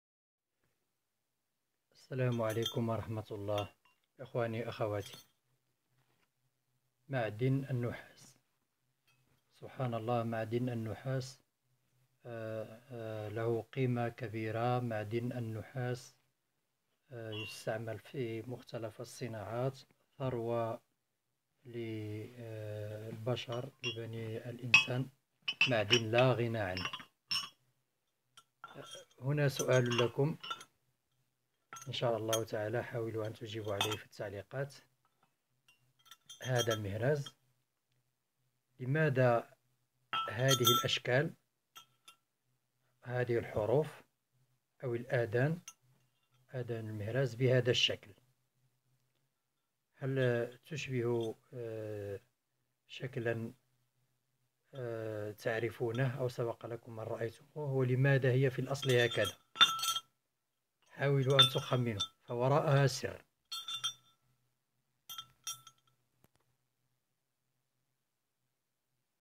سوءال حول المهراز النحاسي
اشتهرت هده المهاريز في بيوتنا وهي تعتبر من الزينة وللعمل ...ولكن هنا ك اسرار معها ترى ما هي#للعلوم#مهراز#نحاس